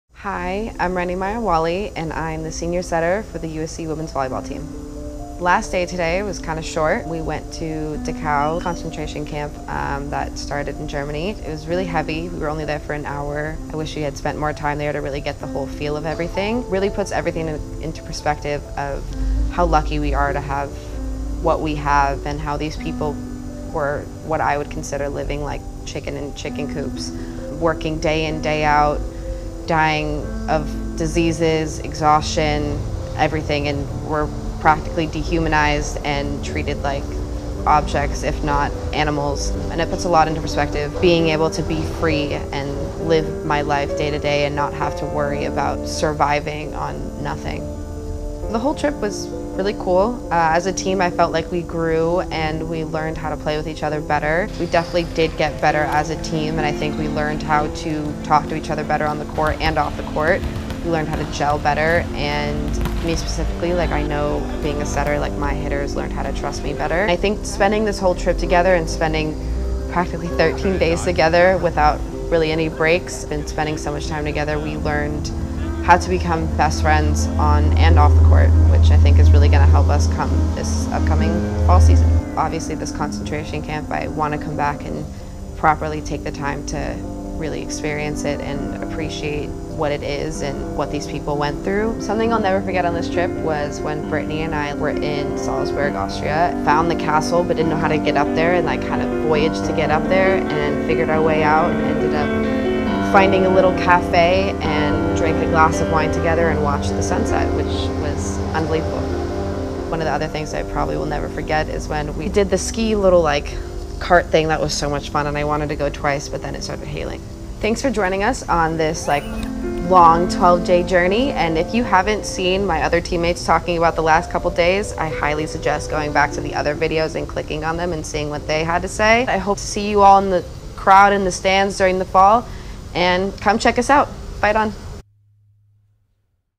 USC Women's Volleyball EuroTrip 2017 - Day 12 :: Dachau, Germany
Senior setter Reni Meyer-Whalley talks about the team's final day abroad.